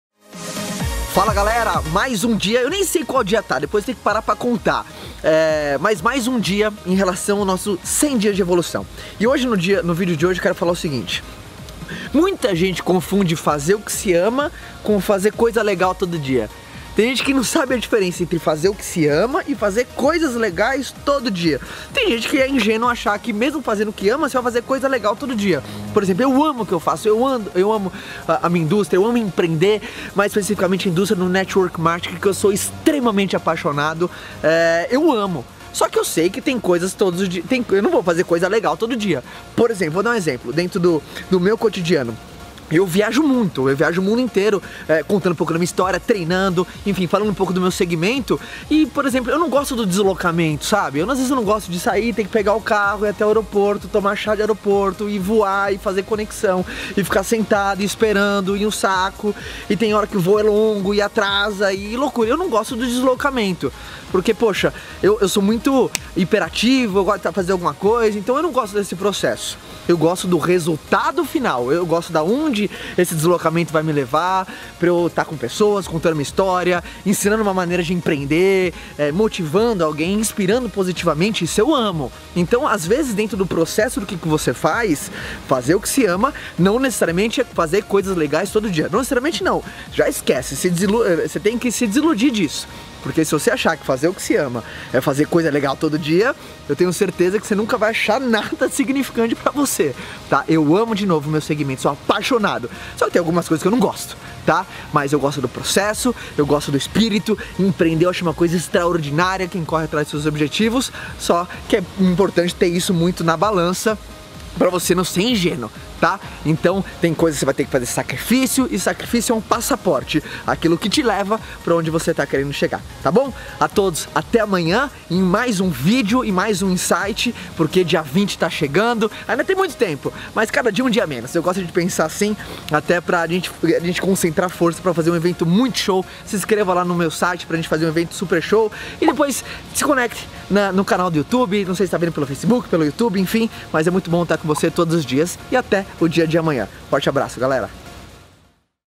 Fala Caio 100DiasDeEvolução - Fazer o que ama, é legal? - 022 de 100
"Fazer o que ama, é legal?"

Vamos fazer um evento online e exclusivo no dia 20 de Agosto após o final dos 100DiasDeEvolução.